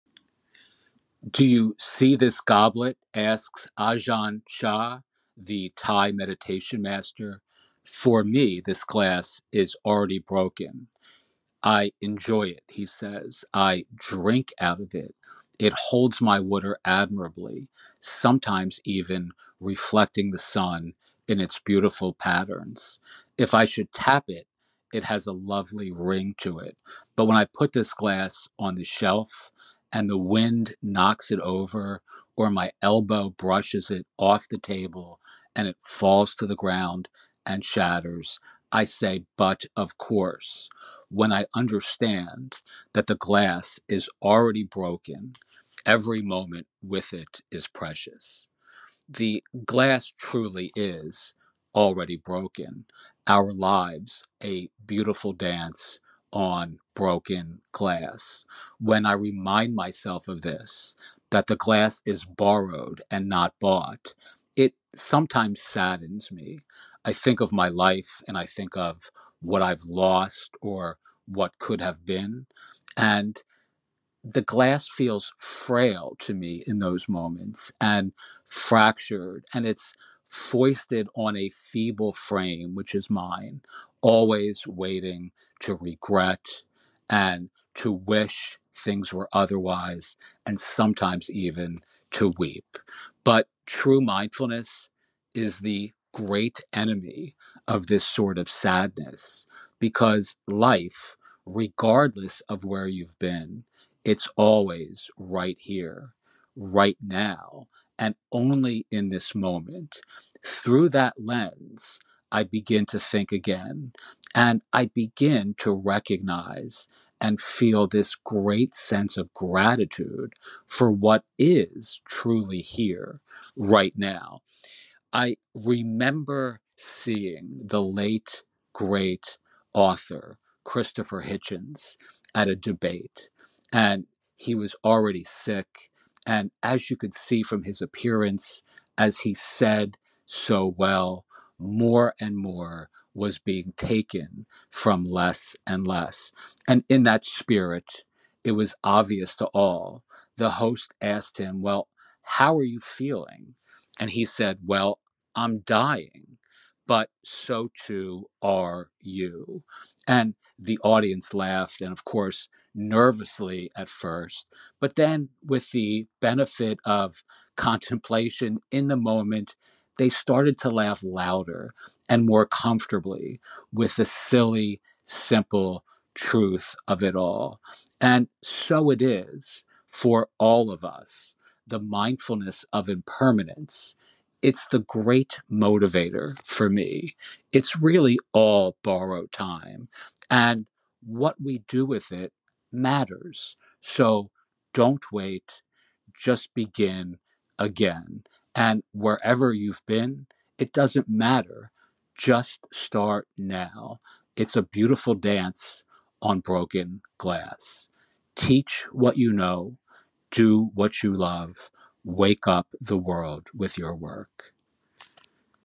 Your Time is Now. The Great Motivator for Starting Today [For Authors, Artists and Entrepreneurs]
So You Want to Teach an Online Course?  A Tiny Course of Big Questions About Creating Fun, Ethical and Profitable Online Courses
Episode 3